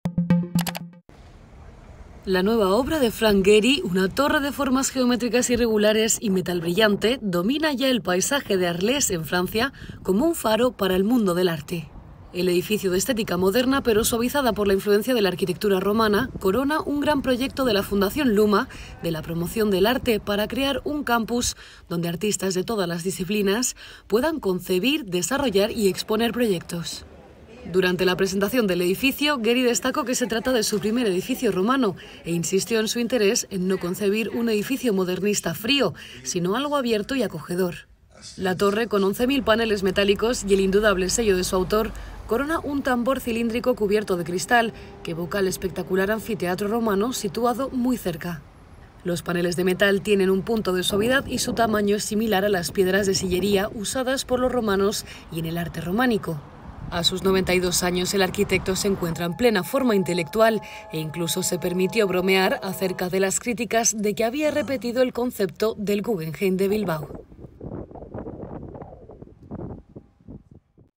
La nueva torre de Gehry brilla bajo el sol de Provenza
Arles (Francia), 25 jun (EFE).- (Imagen: Rafael Cañas) La nueva obra de Frank Gehry, una torre de formas geométricas irregulares y metal brillante, domina ya el paisaje de Arlés, como un faro para el mundo del arte, de estética moderna pero suavizada por la influencia de la arquitectura romana de esta ciudad francesa.